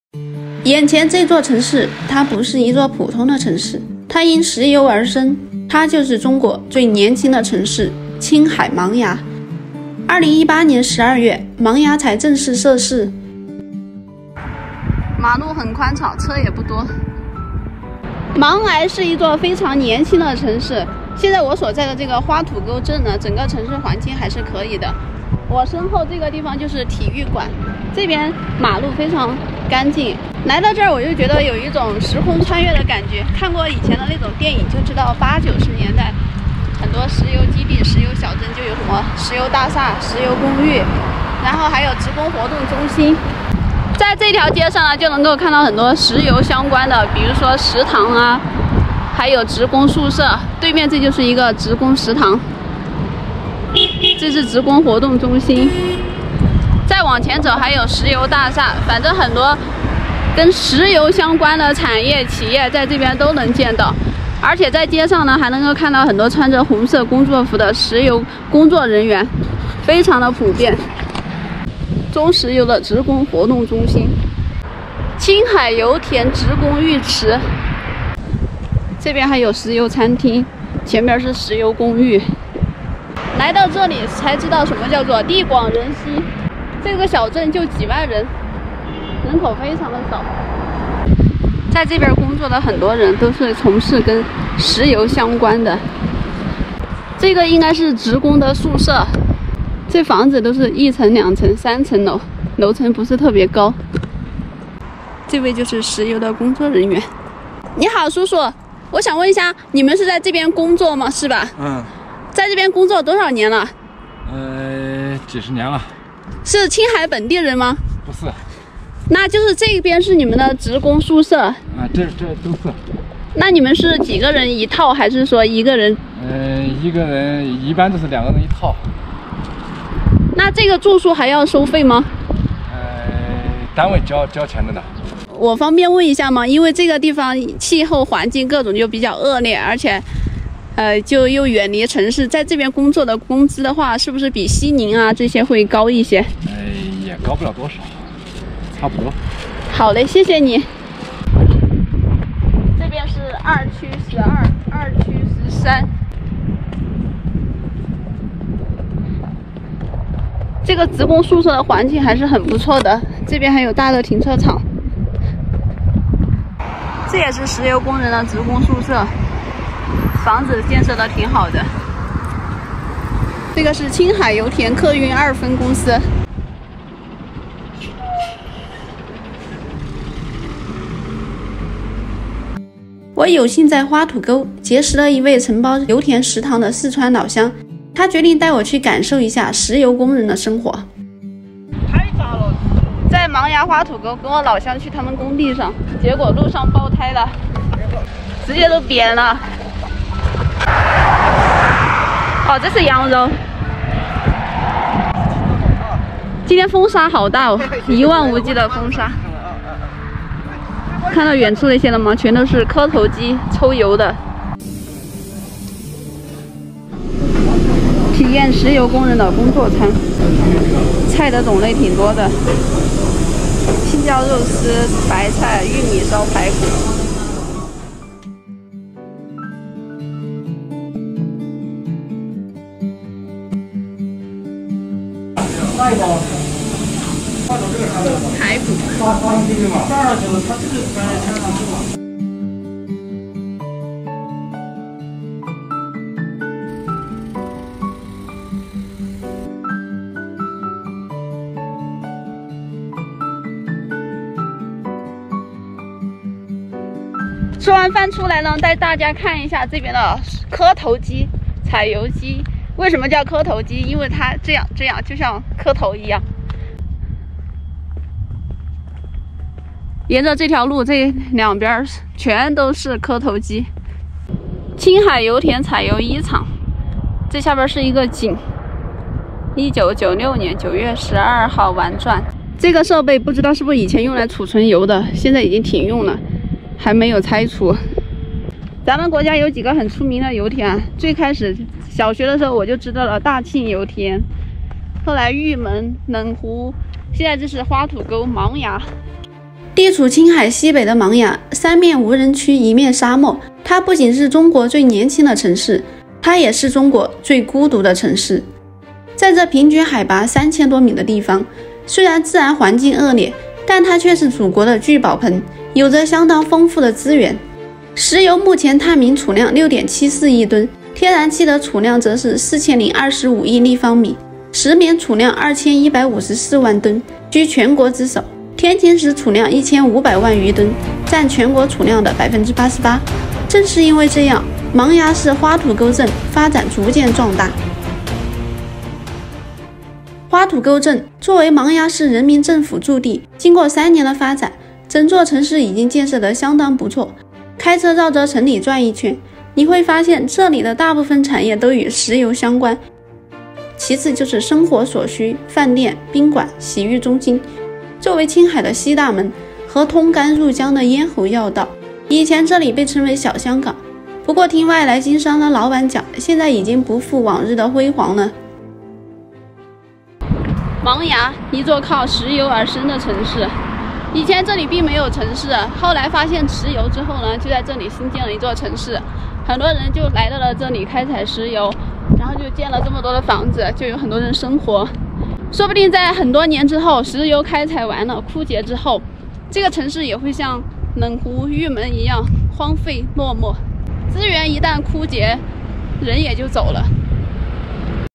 青海这座小镇因石油而生，被称小香港！打工收入很高，你愿意来吗？
青海这座小镇因石油而生，被称小香港！打工收入很高，却要忍受恶劣环境和孤独，你愿意来吗？